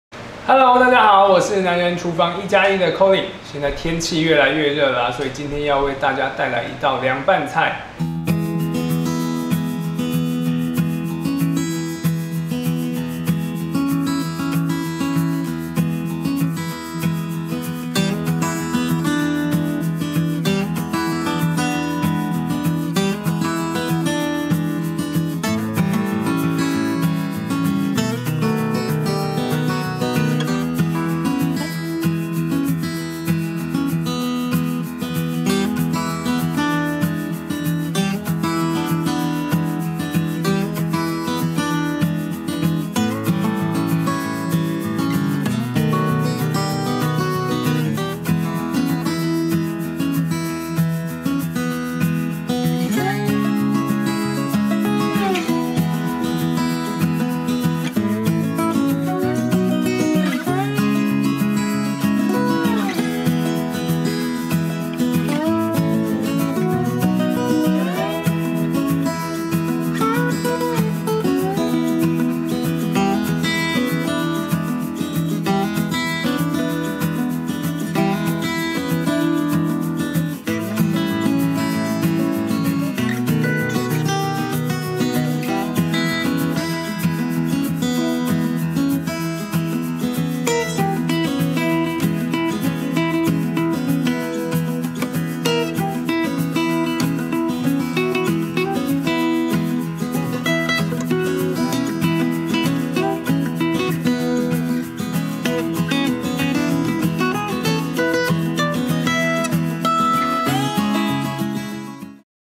開胃界的第一把交椅──韓式涼拌辣醃蘿蔔｜ELLE Cooking
氣溫依舊炎熱的秋初，最適合吃辣醃蘿蔔這道冰涼、清爽、開胃的涼拌菜！